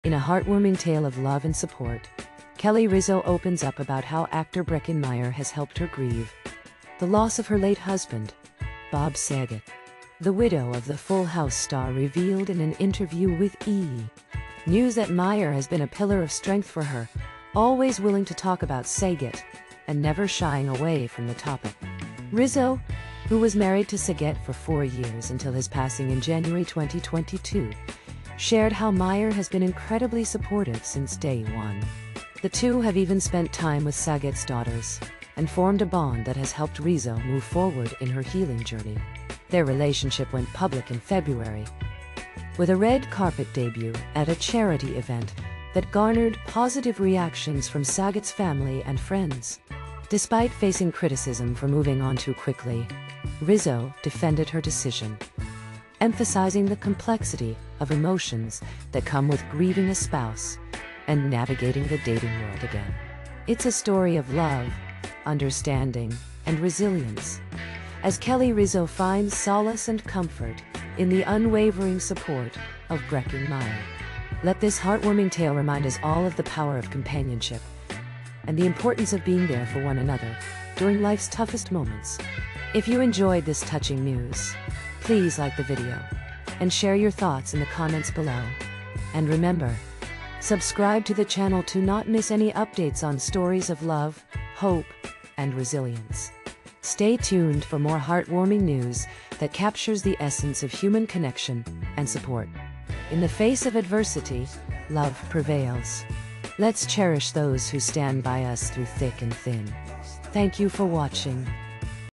Kelly Rizzo Opens Up About Finding Healing with Breckin Meyer | Exclusive Interview
Kelly Rizzo shares her journey of healing after the loss of her husband Bob Saget, and how her relationship with actor Breckin Meyer has been supportive and comforting. Watch the exclusive interview to learn more.